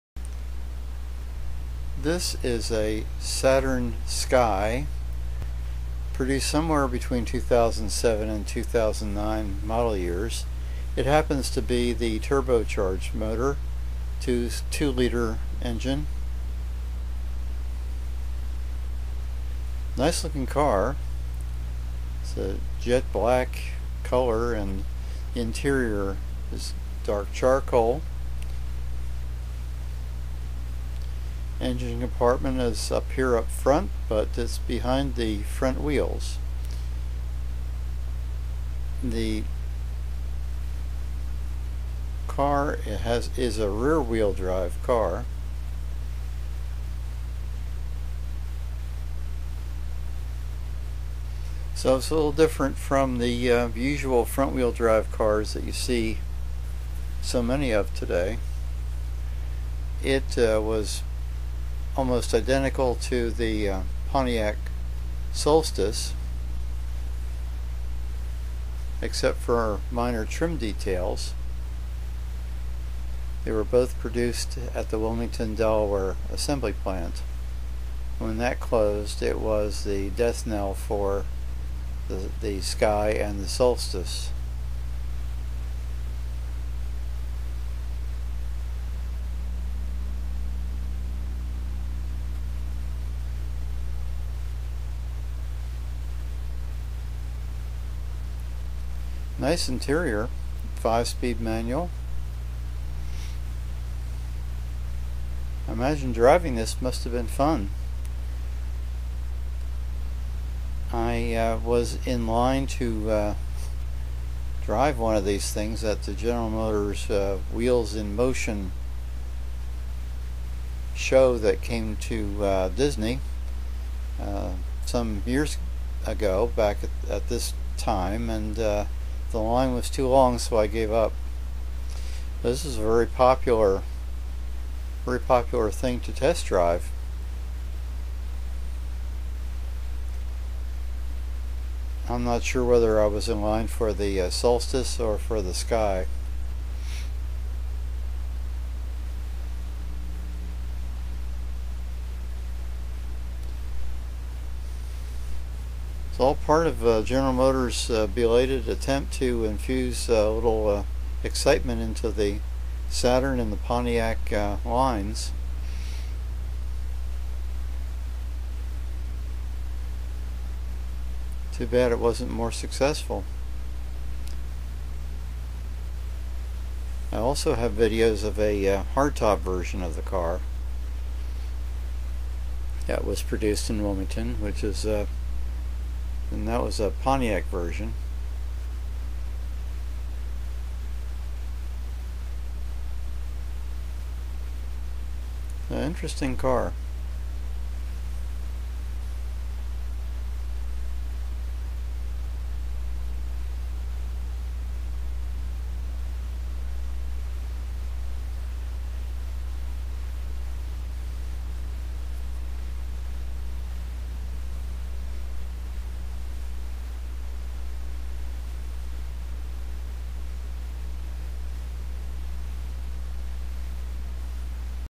Saturn Sky Turbo Convertible Blk Longwood 071313
A very nice example of a turbo Saturn Sky.  It's a pity that it couldn't continue in production.